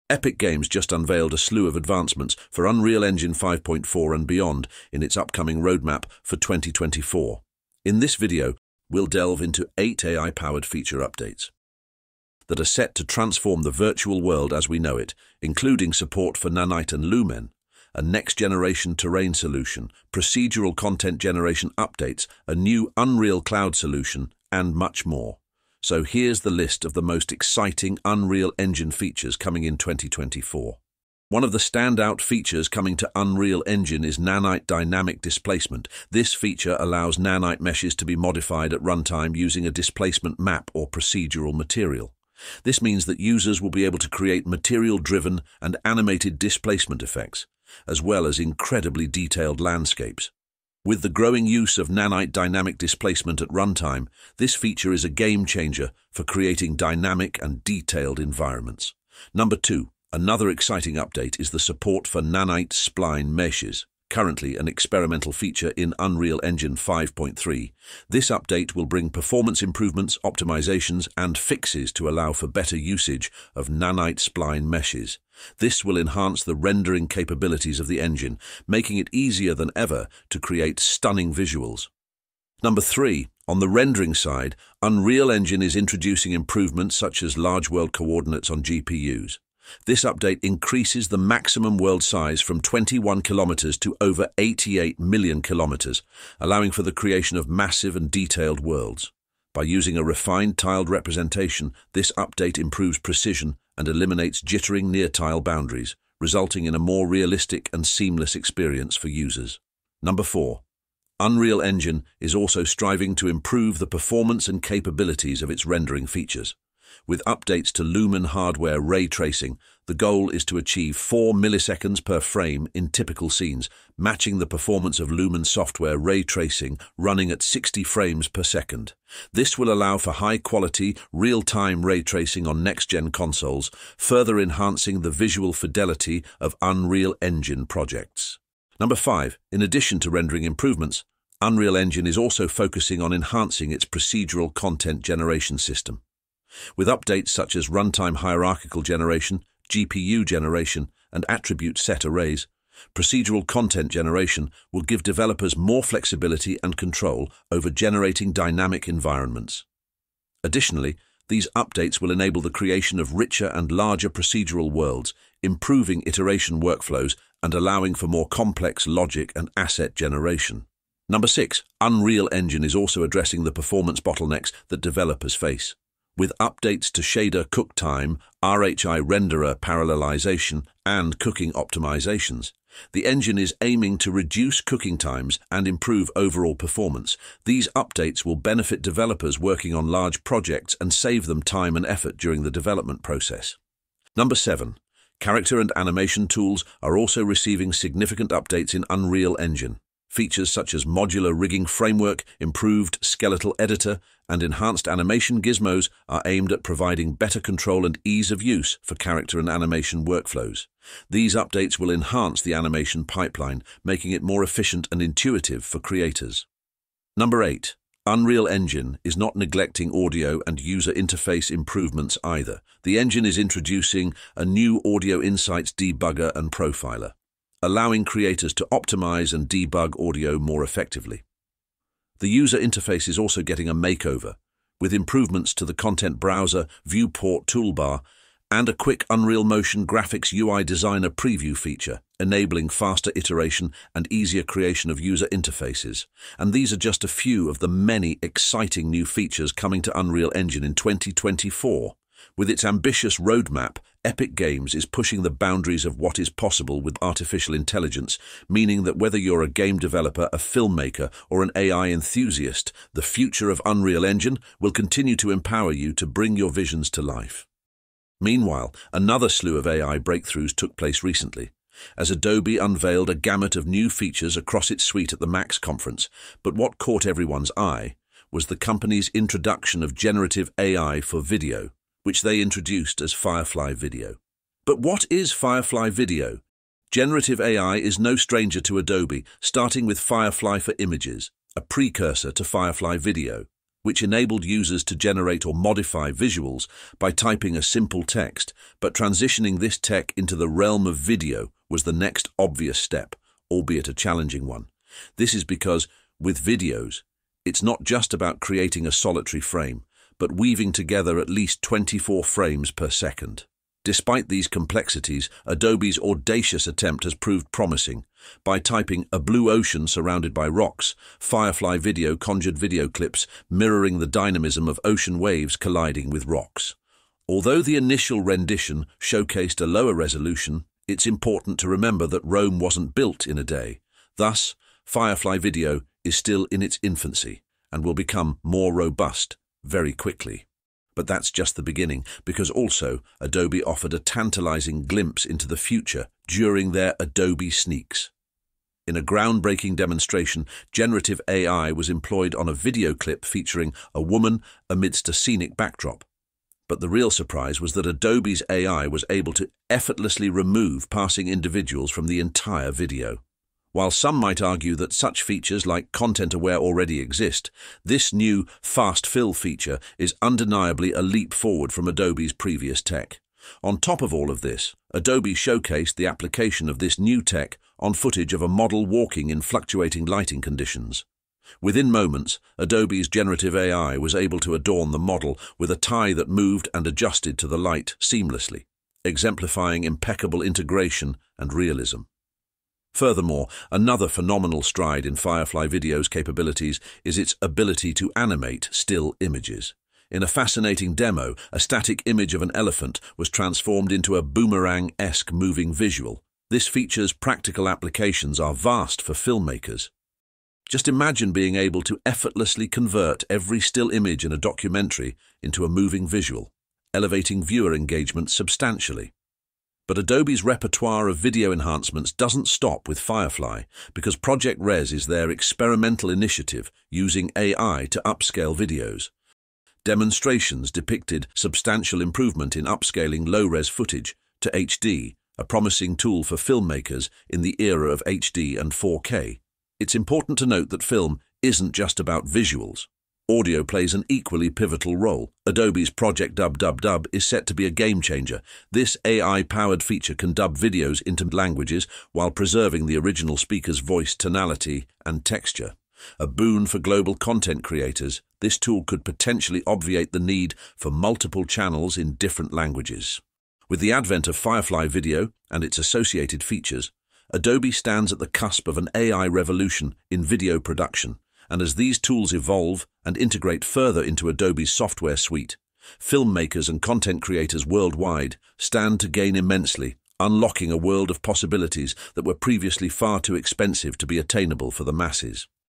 8 Upcoming Unreal Engine 5.4 Features in 2024 | AI Graphics Preview
- Epic Games has unveiled a range of exciting updates for Unreal Engine 5.4 and beyond in its 2024 roadmap, including dynamic displacement for Nanite meshes and performance enhancements, aimed at enhancing creators' capabilities. Adobe introduced Firefly Video, a generative AI tool for video editing, showcasing its potential to transform video production with features like 'Fast Phil' and audio dubbing, promising a revolution in the industry.